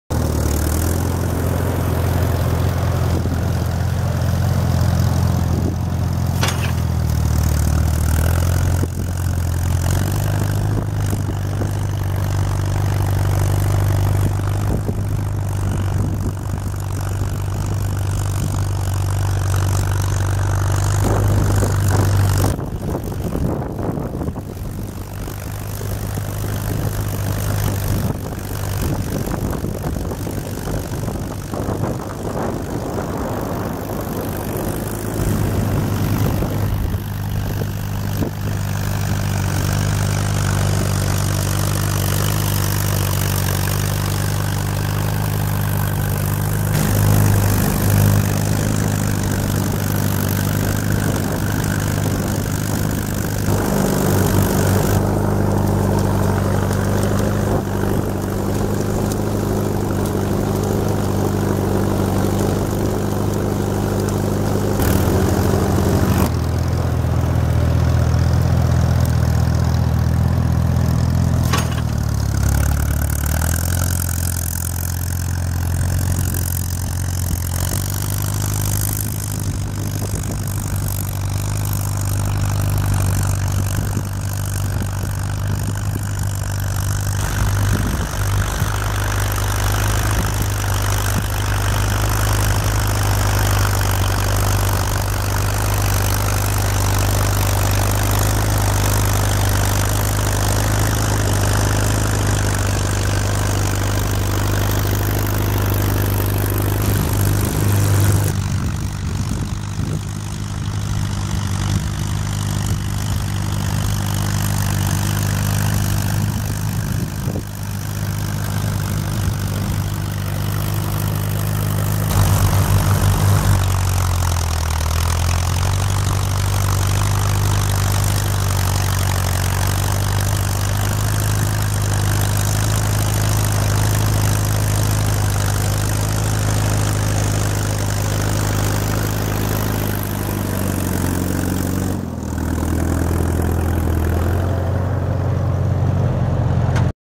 Plowing with an Allis Chalmers WD -- Nov 1st & 5th, 2020
Plowing two separate days with my Allis Chalmers WD, November 1st (in the high winds, where you can see some stuff flapping around on my camera tripod) and November 5th (where the ground has definitely dried out a lot). Plowing in 2nd gear, WOT.